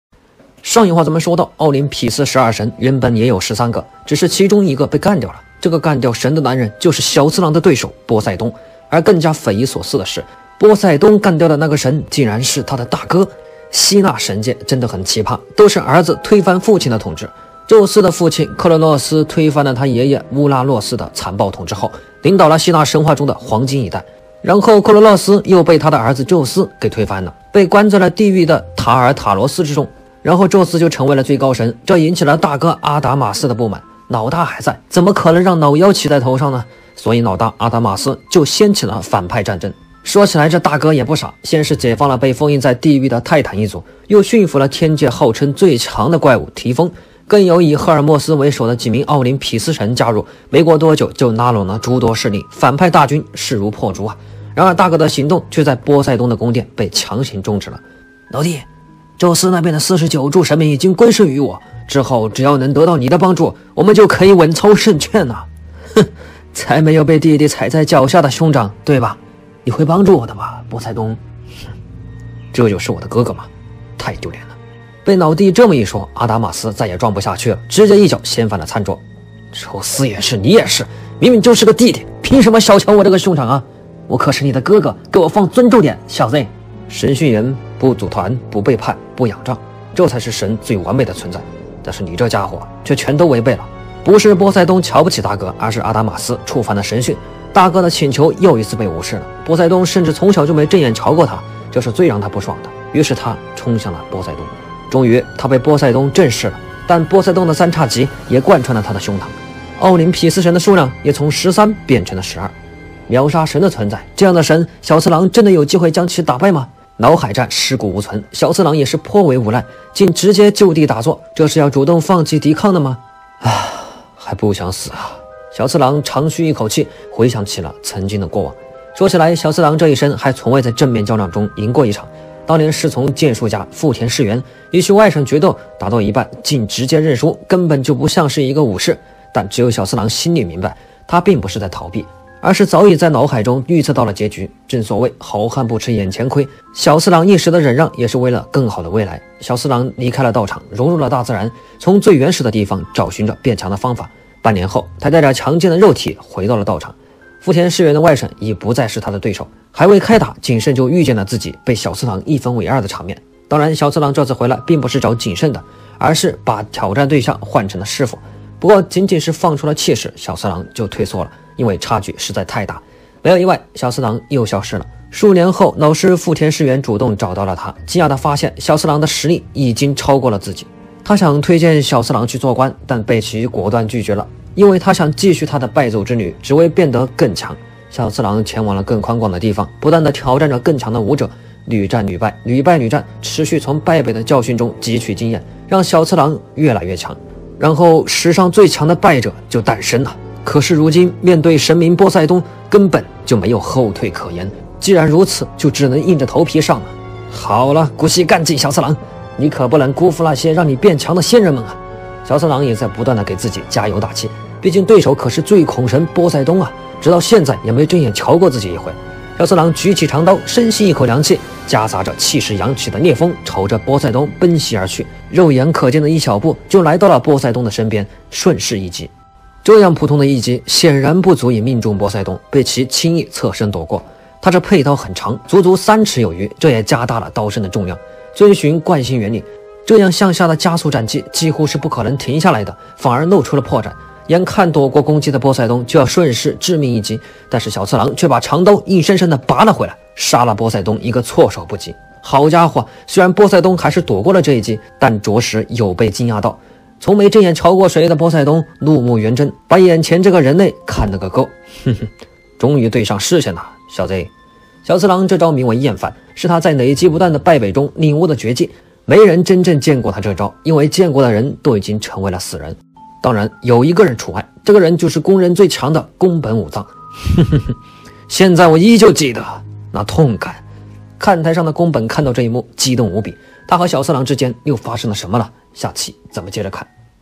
终末的女武神07：神明理发师小次郎，波塞冬刘海被砍
影片来源自: 天师张品漫
西瓜视频:天师张品漫
抖音:天师张品漫

这是一部非常热血的漫画, 人类与神明的大战!